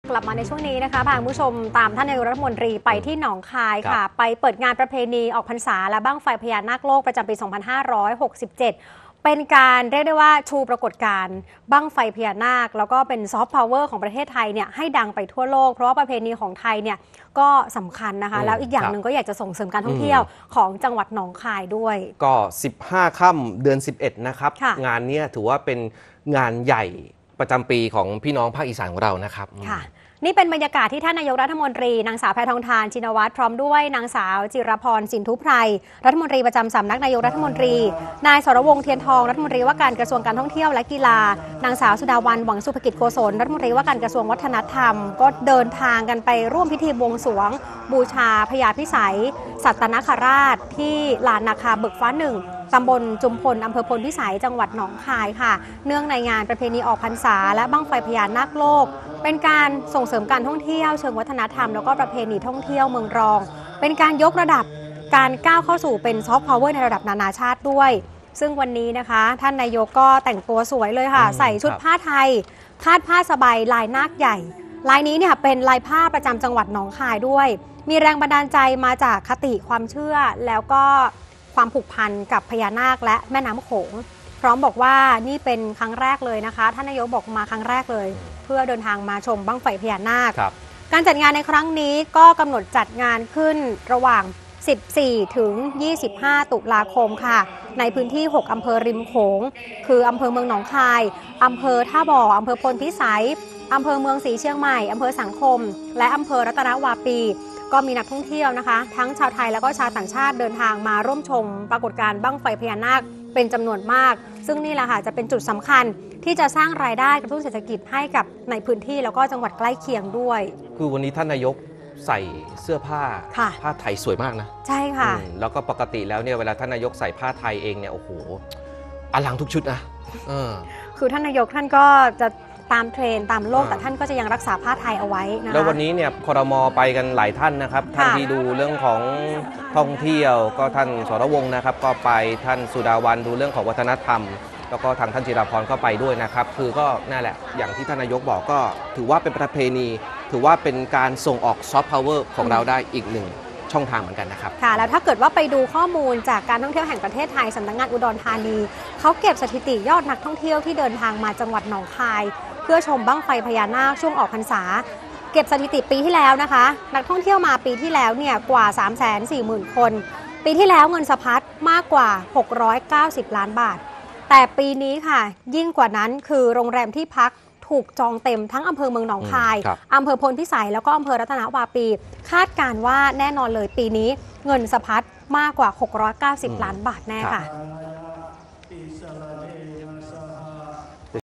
นายกฯ เปิดงานประเพณีออกพรรษาและบั้งไฟพญานาคโลก ข่าวดึก วันที่ 17 ตุลาคม 2567
นายกรัฐมนตรี  เปิดงาน“ประเพณีออกพรรษาและบั้งไฟพญานาคโลก” ประจำปี 2567 ชูปรากฎการณ์บั้งไฟพญานาค ซอฟต์พาวเวอร์ของประเทศไทยให้ทั่วโลกรู้จักประเพณีของประเทศไทย  ส่งเสริมการท่องเที่ยวของ จ.หนองคาย